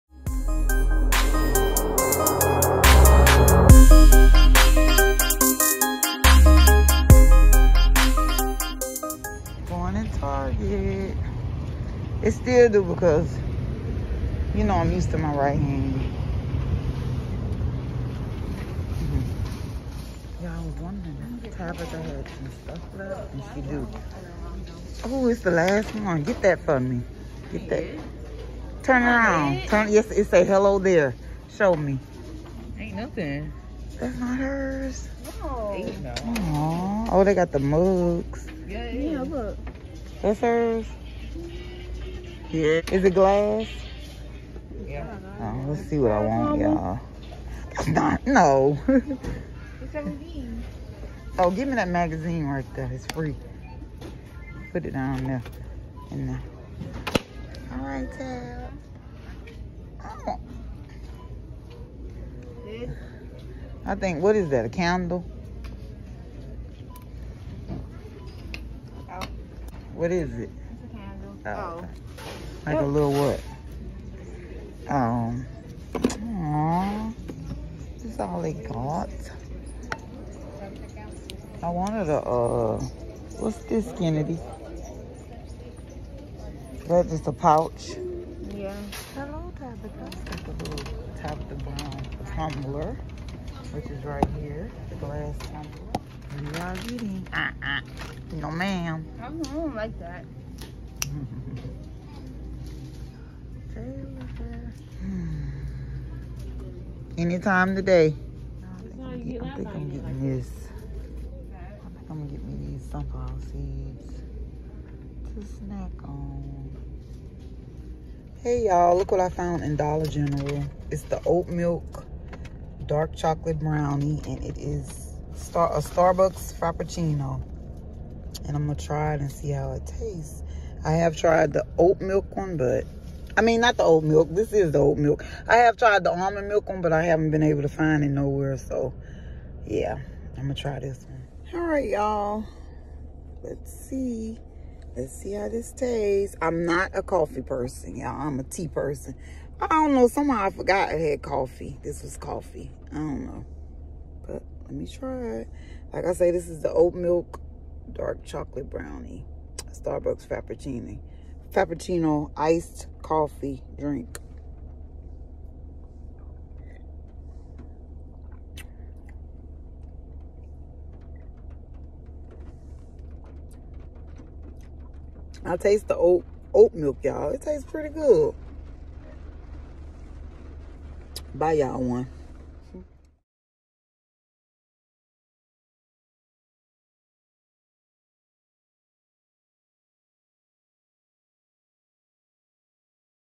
Vlog| Some Of Tabitha Brown’s Second Collection @Target|Shopping With Teens|Taste Test
playing in this video